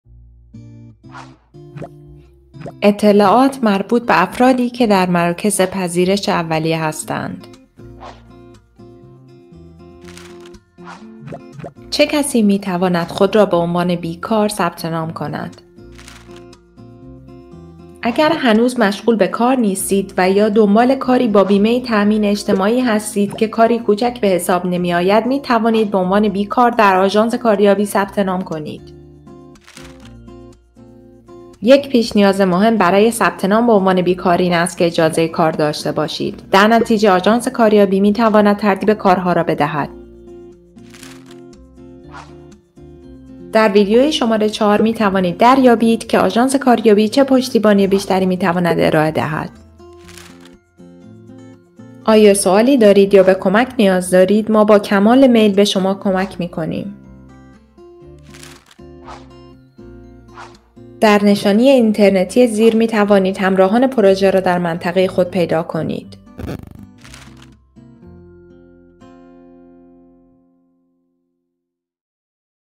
3 چه کسی می‌تواند خود را به عنوان بیکار ثبت نام کند؟
Im Rahmen des Projekts Netwin 3 stellen wir Informationen für  Asylbewerber*innen und Geflüchtete zur Verfügung, mit denen wir sie bei der Arbeitsmarktintegration unterstützen wollen.